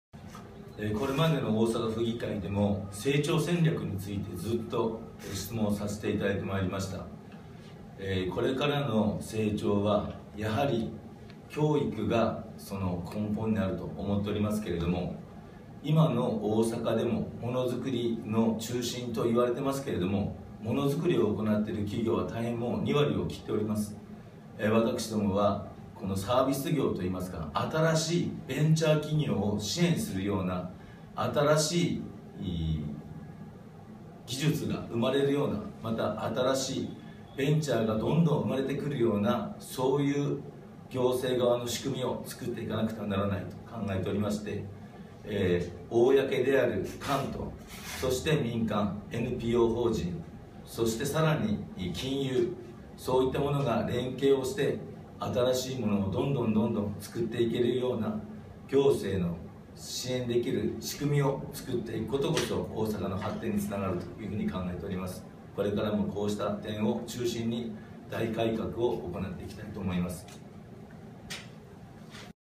e-みらせん 大阪府議会選挙 やえがし善幸候補 設問４
設問４　政策について
撮影・映像制作　大阪ブロック協議会・豊中青年会議所